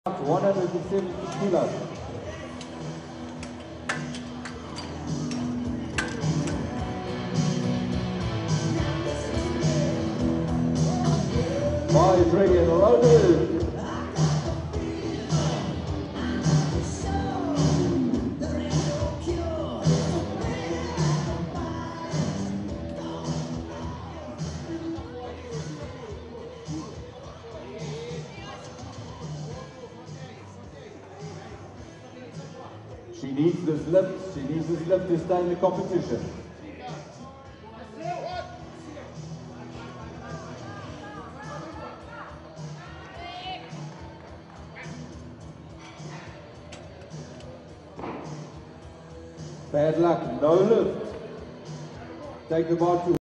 Lurdes Clemente - Squat (mod video)
31th IPF Women's World Championships
South Africa, Potchefstroom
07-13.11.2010